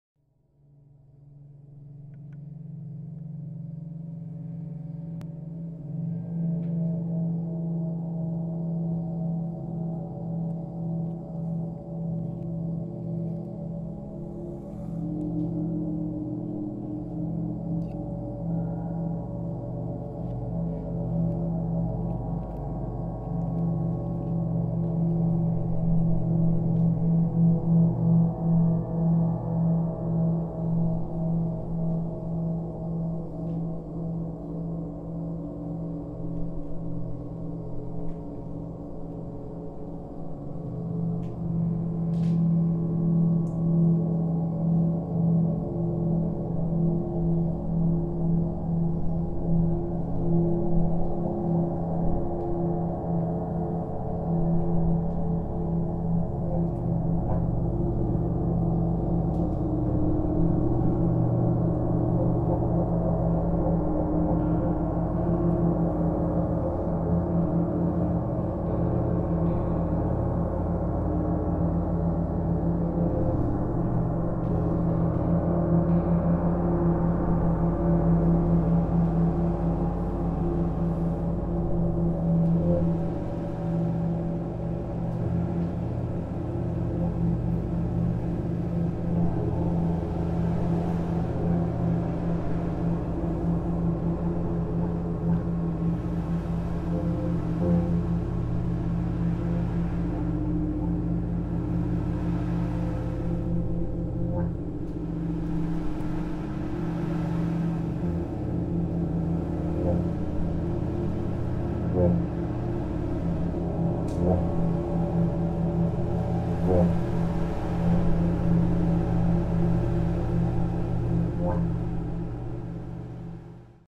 Mindfulness in Corporations
Presenting how the sound of gongs and Himalayan singing bowls can positively effect mindfulness on a corporate level.